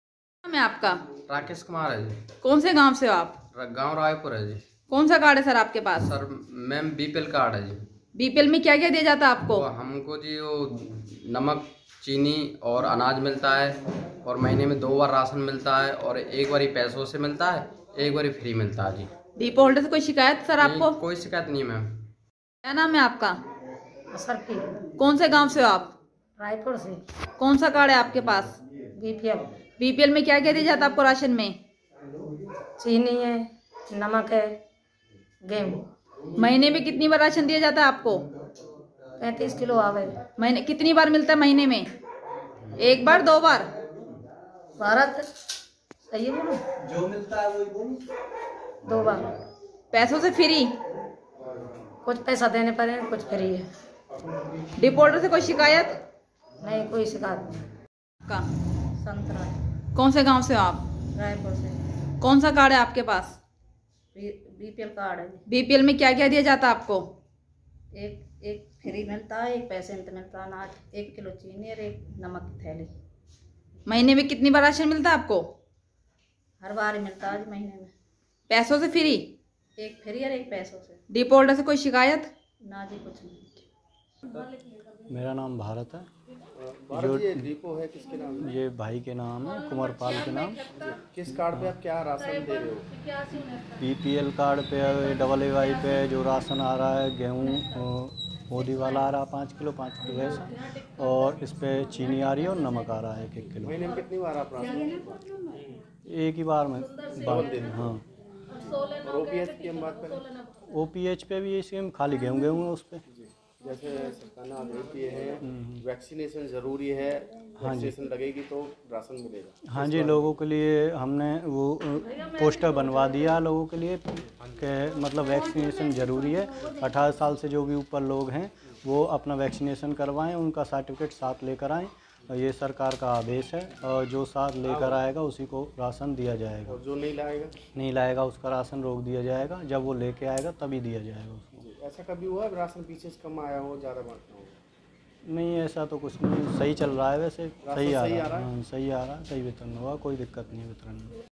पलवल के रायपुर गांव में किस तरीके से राशन वितरित किया जा रहा है देखिए
पलवल के रायपुर गांव में किस तरीके से राशन वितरित किया जा रहा है देखिए ख़ास रिपोर्ट पत्रकार पूजा सैनी के साथ